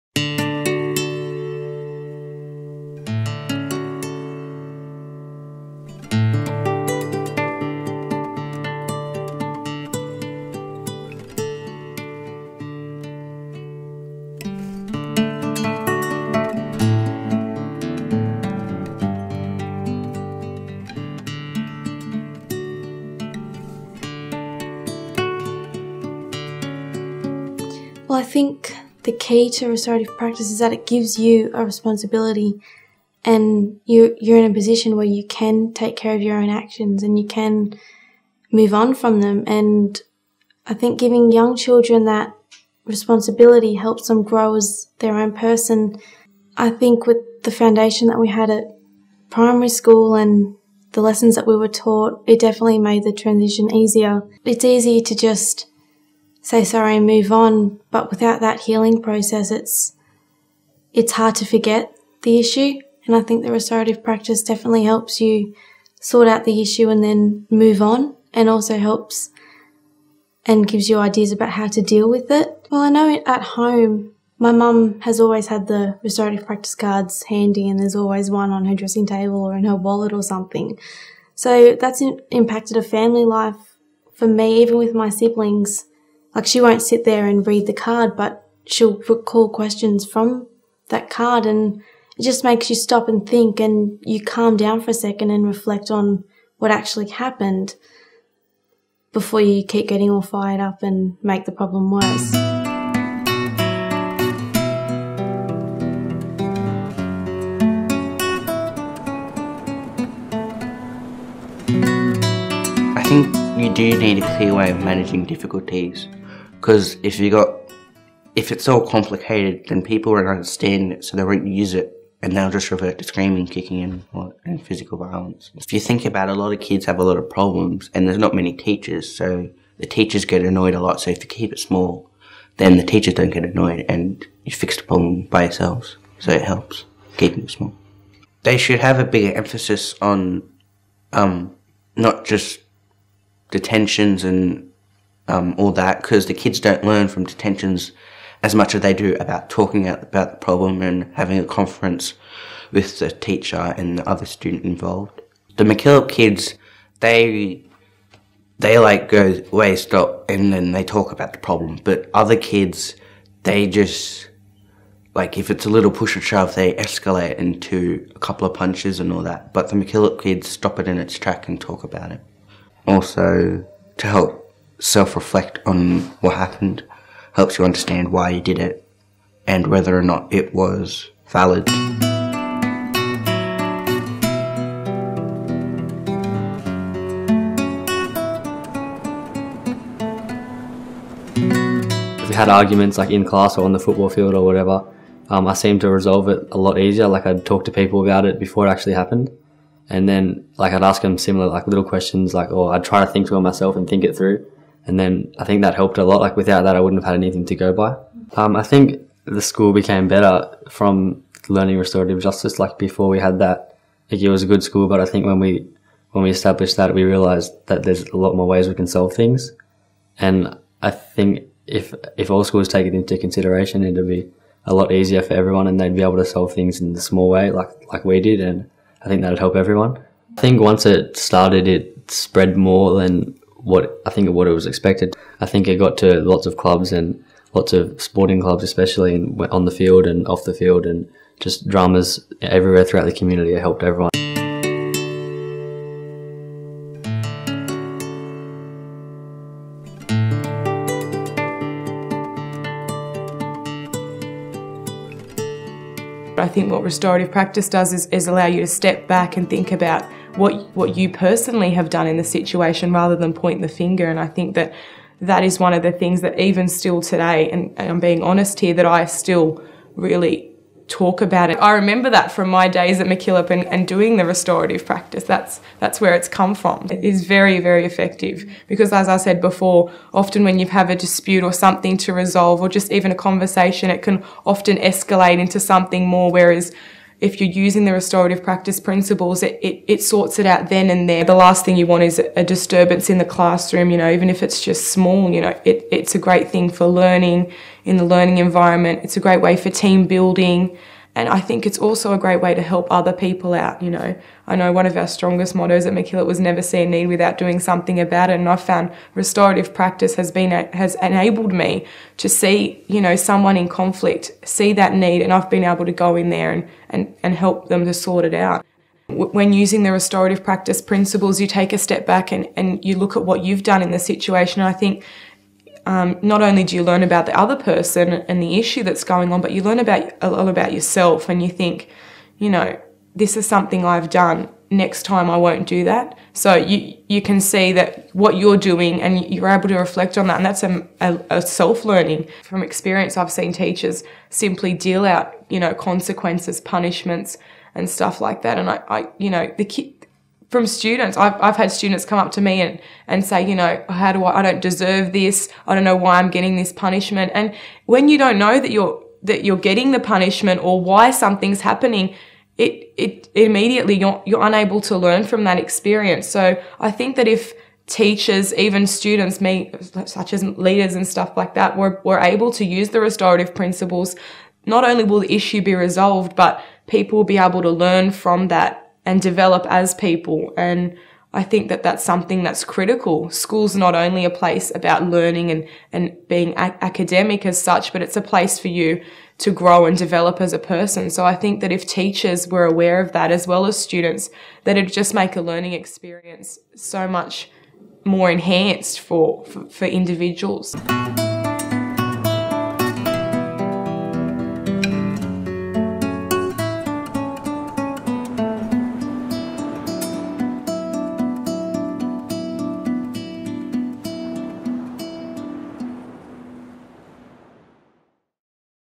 Restorative Practices in High Schools
Four students from the pilot school talk about their experiences of being Restorative students during their days in high school.